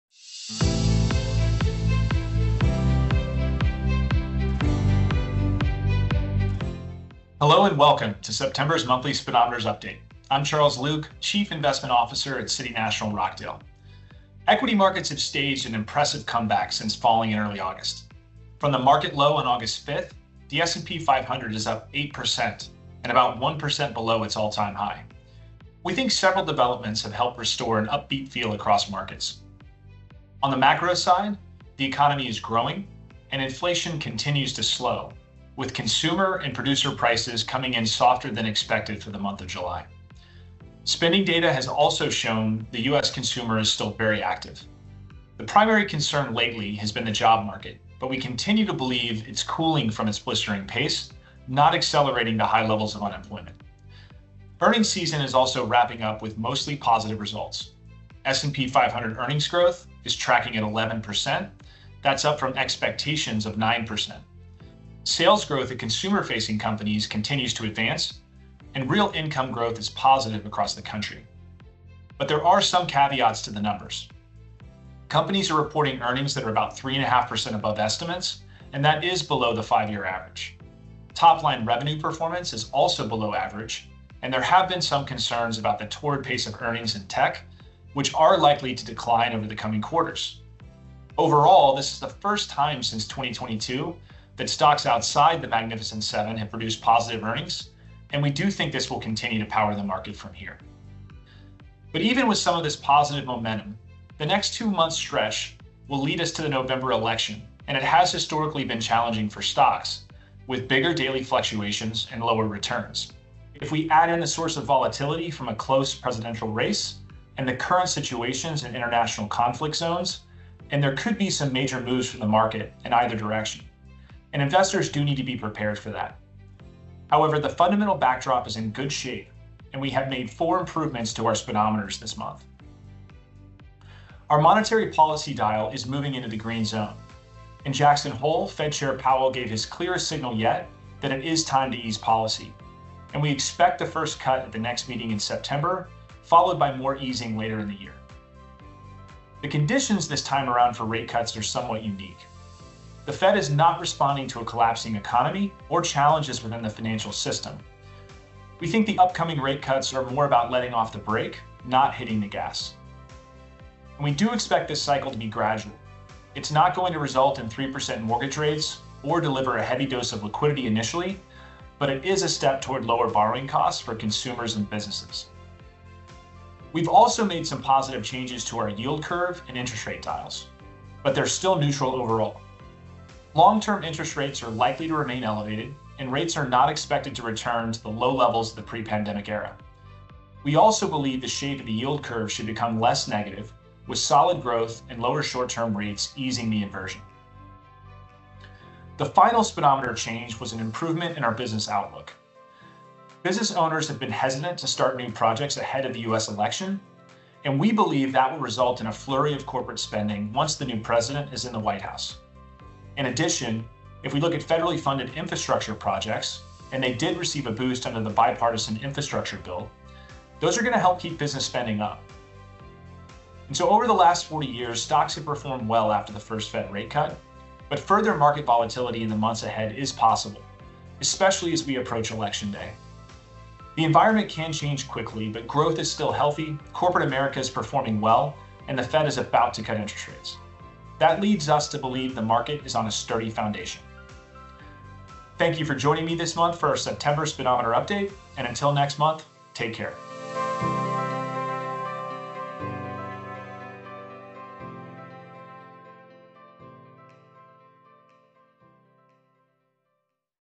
CNR Speedometers: September 2024
Watch Charles Luke, Chief Investment Officer, in our latest video commentary discussing our proprietary monthly global economic and market summary indicators, CNR Speedometers®. These indicators are an important component of our ongoing commitment to keeping clients informed.